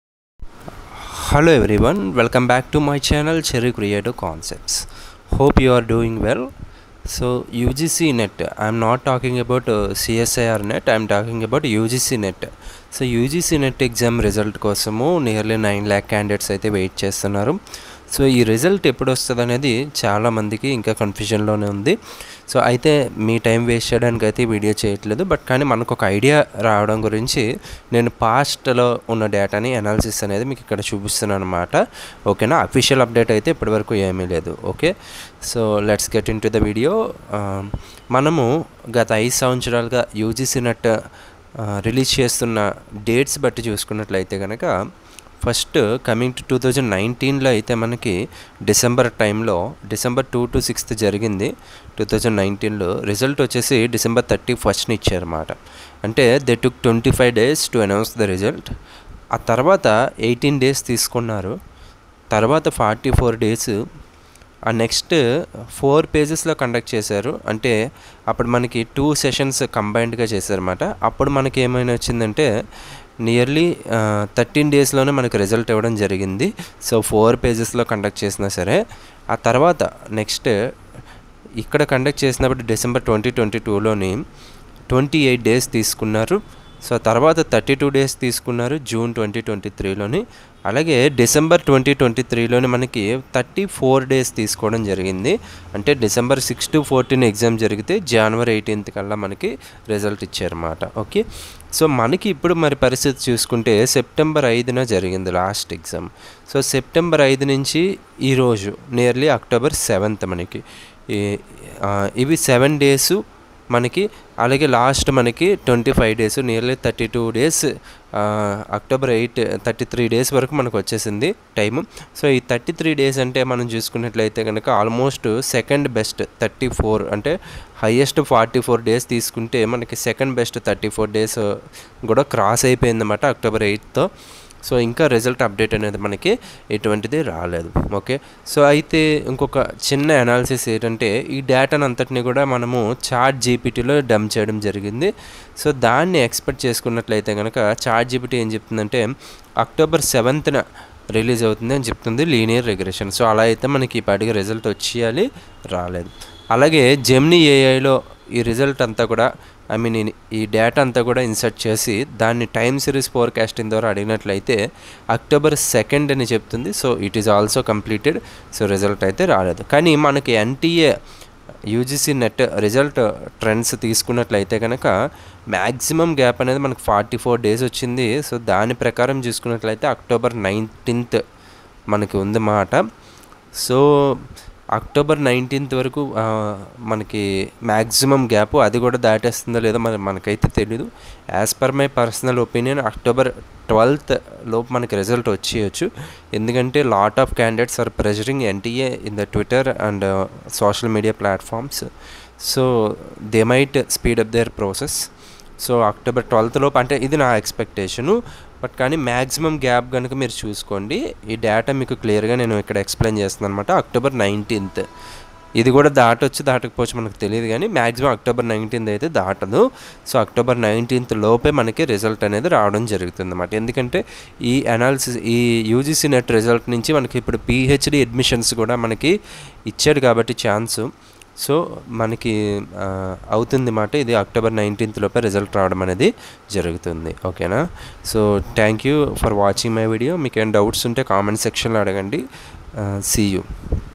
NTA UGC NET 2024 Result expected date | Latest News on UGC net result date | important information
Welcome to Cherry Creative Concepts! In this video, we’ll be discussing the expected result date for the NTA UGC NET 2024. If you’ve been eagerly waiting for the results or are curious about the latest news on UGC NET updates, this video is for you! We’ll cover the official announcements, important information for candidates, and tips on how to stay informed and prepared.

Key Points Covered:

NTA UGC NET 2024 result expected date
Latest news and updates regarding the UGC NET result
Important information for UGC NET candidates
Your support helps us create more informative content for you!